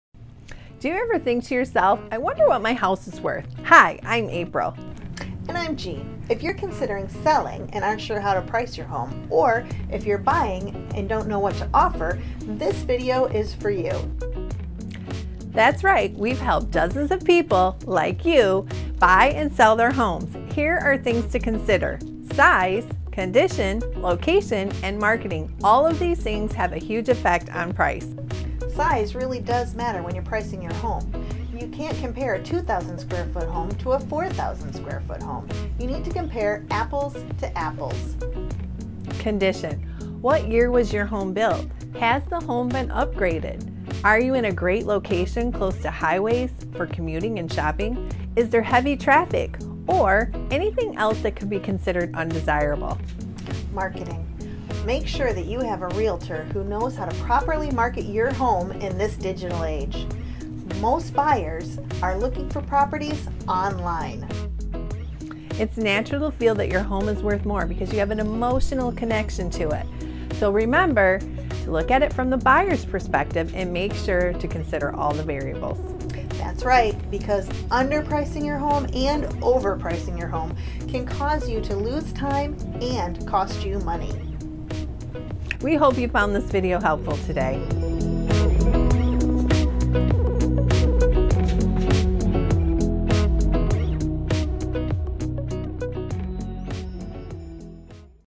What Is Your Home Worth?
Do you ever think to yourself? I wonder what my house is worth?

If you're considering selling and aren't sure how to price your home, or if you're buying and don't know what to offer this video is for you.  That's right, we've helped dozens of people like you, buy and sell their homes. Here are things to consider. Size, condition, location, and marketing. All of these things have a huge effect on price.

 Size really does matter when you're pricing your home. You can't compare a 2000 square foot home to a 4,000 square foot home. You need to compare apples to apples.

Condition. What year was your home built? Has the home been upgraded? Are you in a great location close to highways for commuting and shopping? Is there heavy traffic? Or anything else that could be considered undesirable.

Marketing. Make sure that you have a realtor who knows how to properly market your home in this digital age. Most buyers are looking for properties online.

It's natural field that your home is worth more because you have an emotional connection to it. So remember to look at it from the buyer's perspective and make sure to consider all the variables.

That's right, because under pricing your home and overpricing your home, can cause you to lose time and cost you money.

- We hope you found this video helpful today.